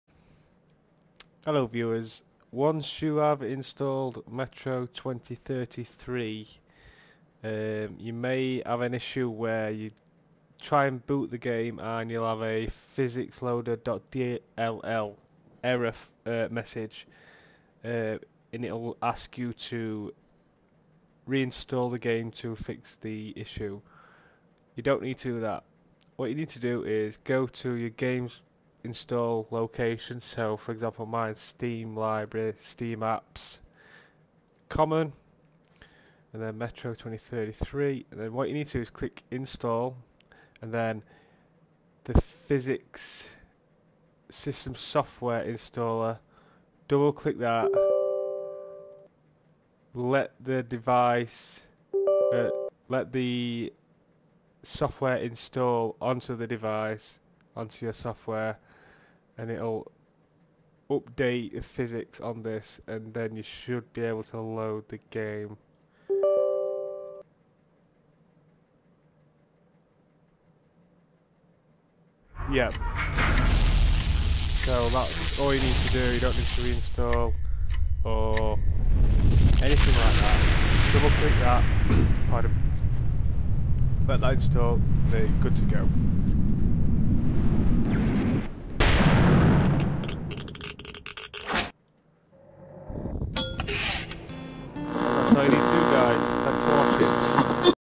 How To Fix PhysicsLoader DLL error message for Metro 2033
Step 1:-
Locate your game install path, for example my path is
This PC - Local Disk (G:) - SteamLibrary - SteamApps - Common - Metro 2033 - Install

Step 2:-
Once you have opened the 'Install' File, you should find a program in there named
PhysiX_10.02.22

Step 3:-
Double Click 'PhysiX_10.02.22  and run the program

Step 4:-
Once you have run the PhysiX program, you will be good to go.